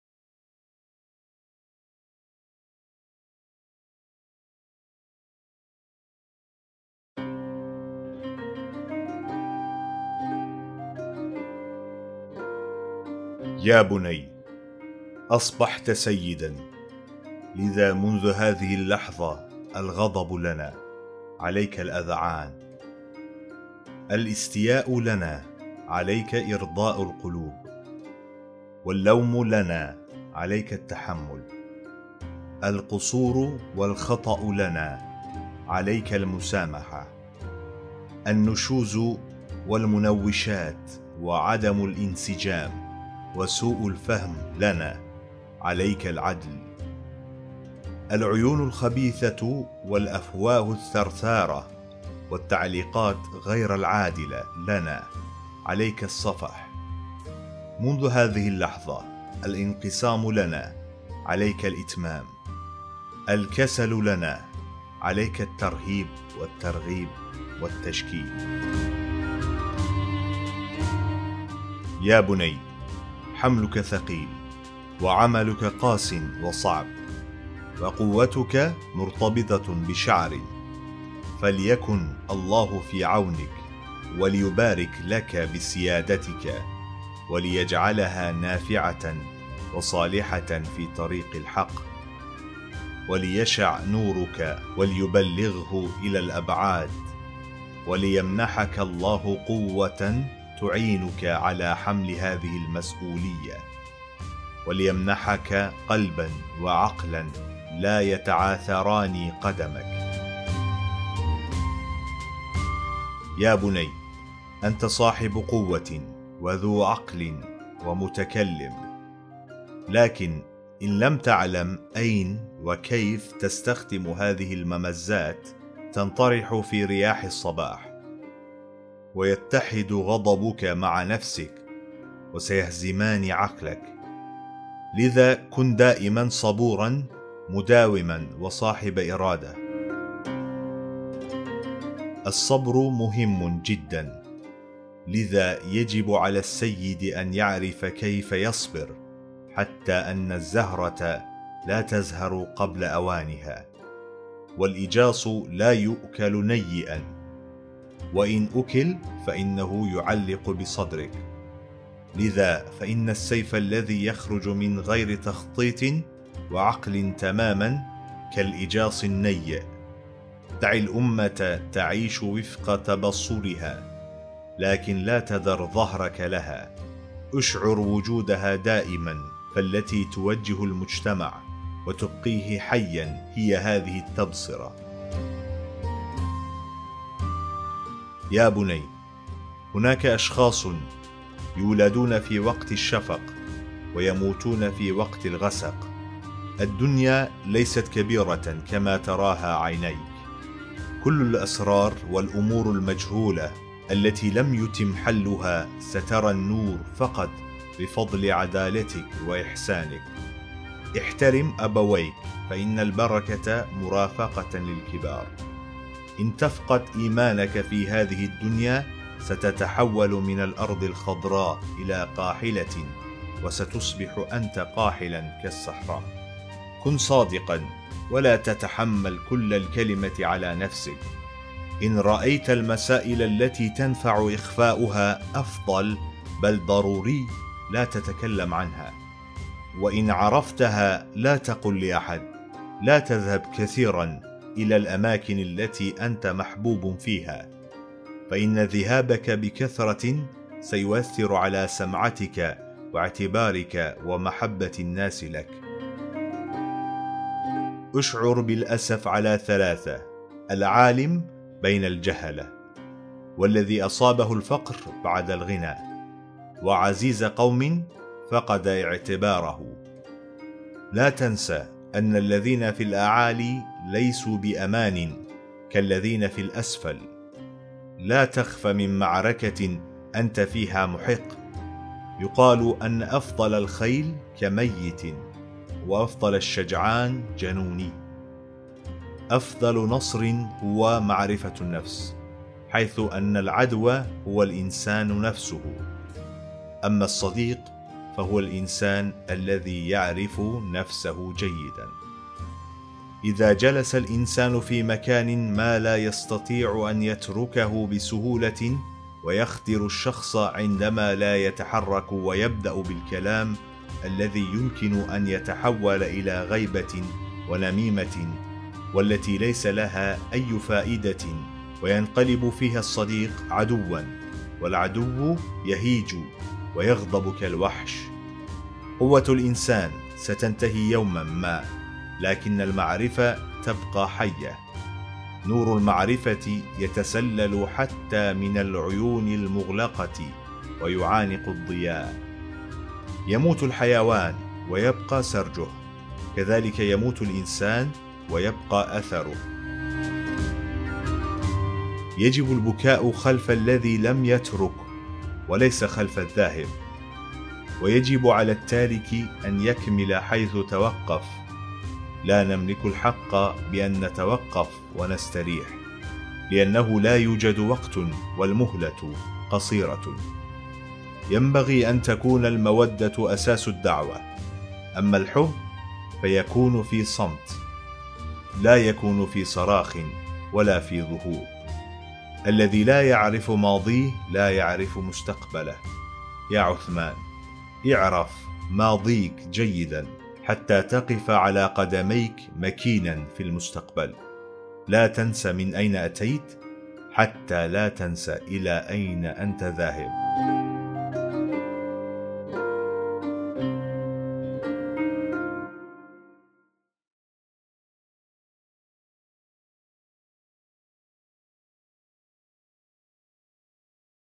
Şeyh Edebâli’nin Osman Gazi’ye Nasihatı - نصيحة الشيخ إده بالي لعثمان | Arapça-Türkçe Hitabeler 11
Hitabeler kitabımızda yer alan onbirinci hitabe, Şeyh Edebâli’nin Osman Gazi’ye nasihatı. Arapça seslendirme, görsel ve Türkçe altyazı ile her hafta bir hitabe yayınlamaya devam ediyoruz. Her Cumartesi 18:00'ı bekleyin.

Kitap: Arap-Türk Edebiyatından Arapça-Türkçe Hitabeler
Hitabe: Şeyh Edebâli
Türkçesi: Kamile Özdemir
Seslendiren: Muhammed Yusuf Halıcıoğlu